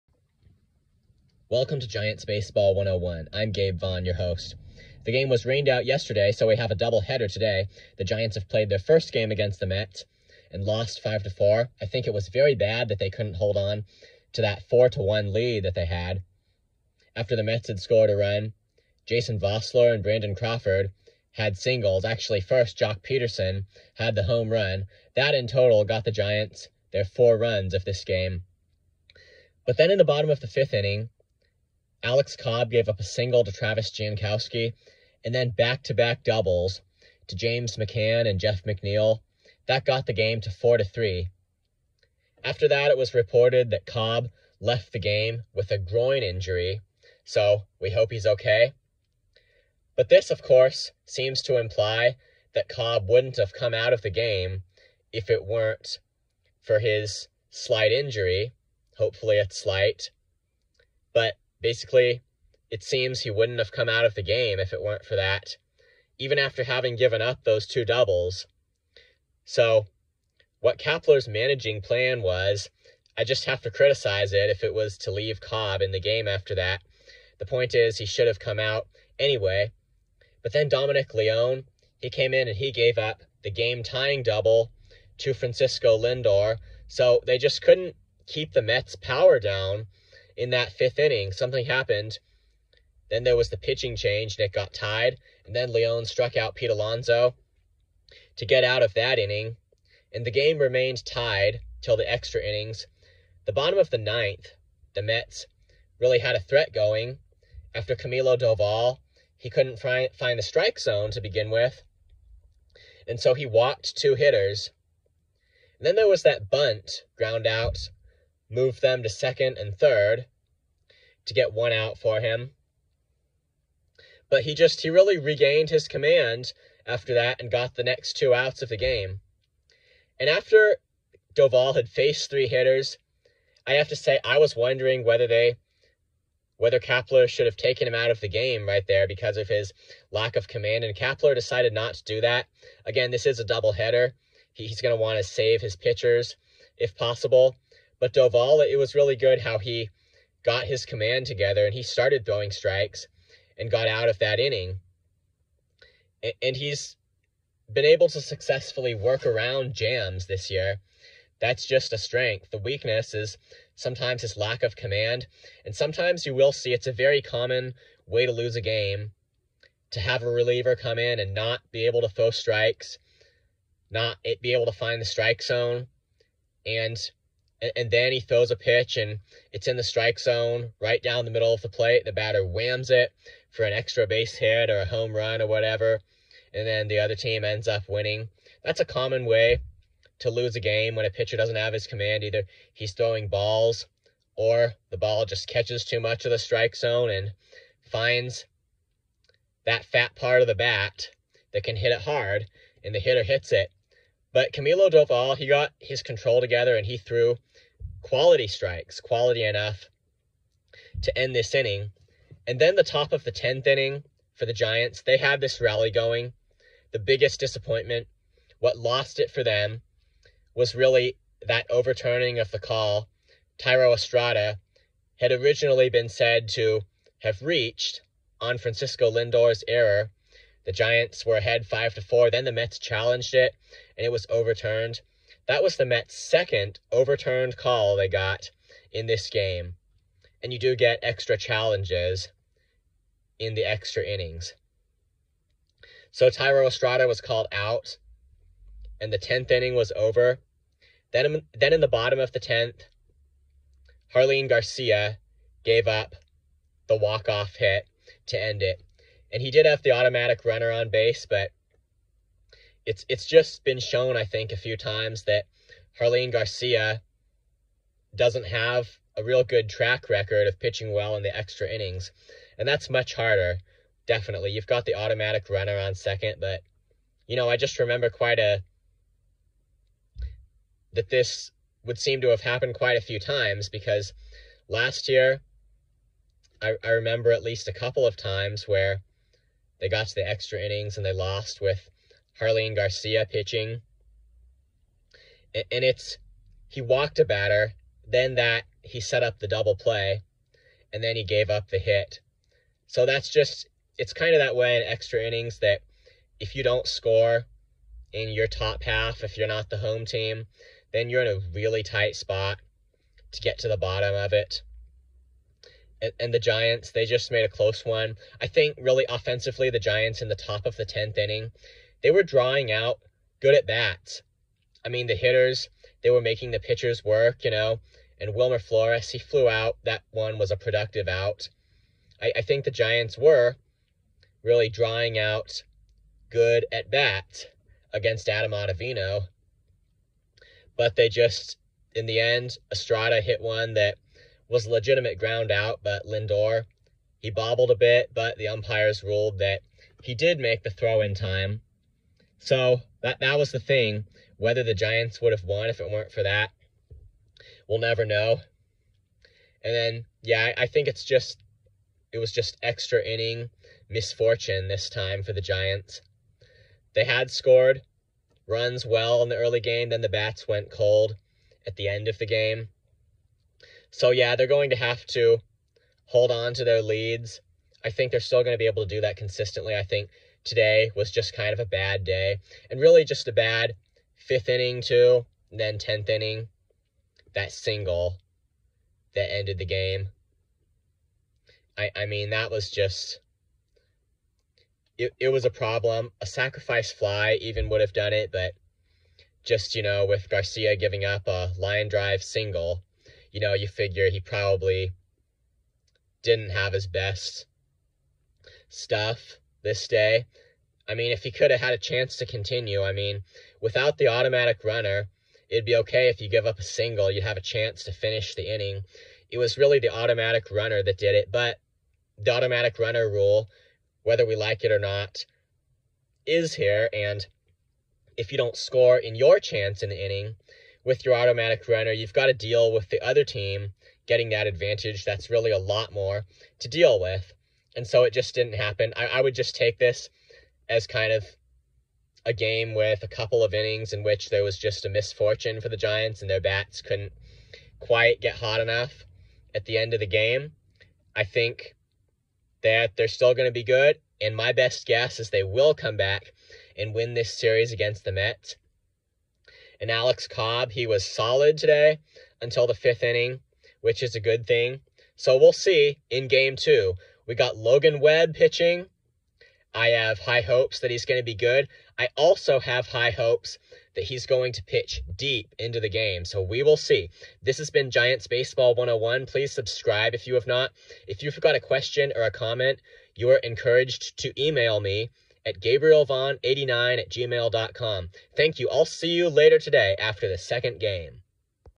They shouldn't have blown this one.
The Giants couldn't figure a way around the Mets' bats in the 5th inning, and couldn't strand the automatic runner in the 10th.